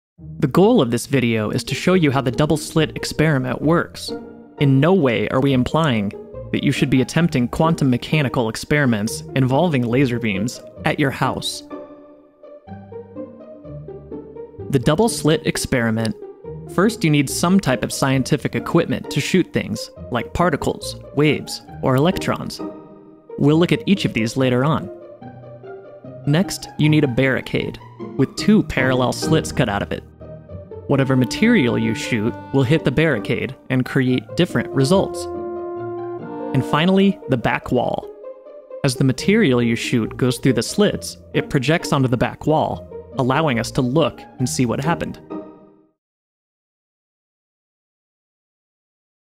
Double-Slit Experiment Explained Part 1: Experiment Setup
The double-slit experiment may seem simple, but it has produced some of quantum mechanic's weirdest results. Many of the findings still leave the science world puzzled, even today!

To understand how the double-slit experiment works, we will start by learning how to set up and prepare the experiment in this part 1 of 5 video.

For more detailed information about the double-slit experiment, read the article now at: